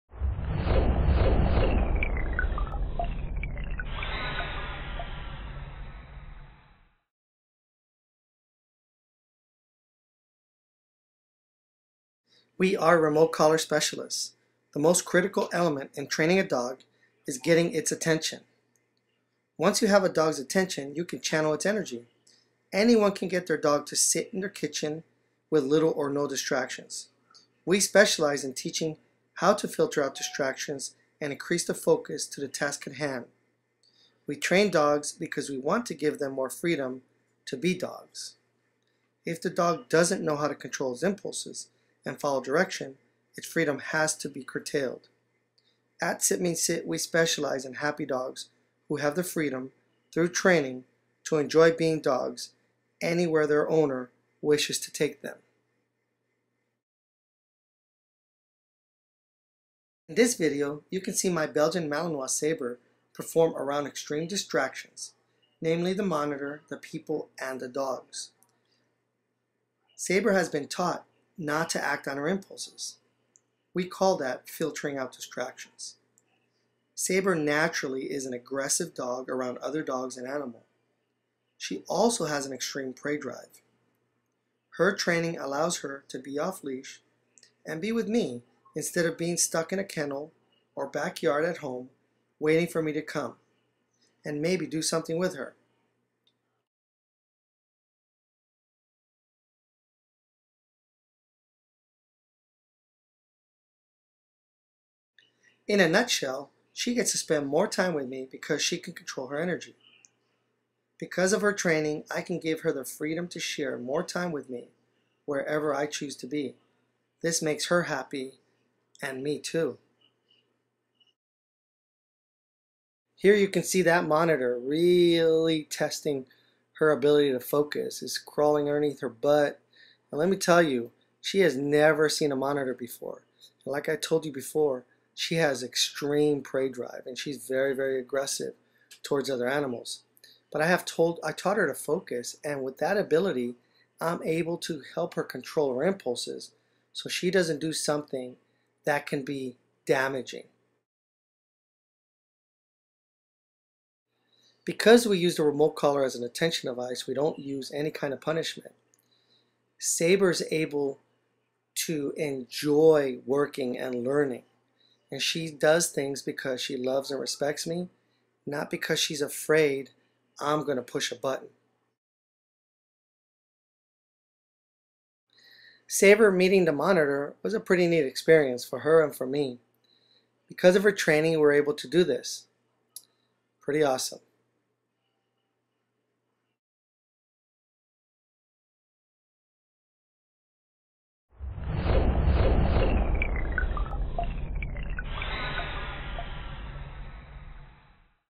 Saber and the monitor
Alfredo Rivera, a Sit Means Sit dog trainer with his dog Saber during a demonstration at a public library event. Saber, a belgian malinois, demonstrates her ability to filter out distractions and perform no matter what the distraction!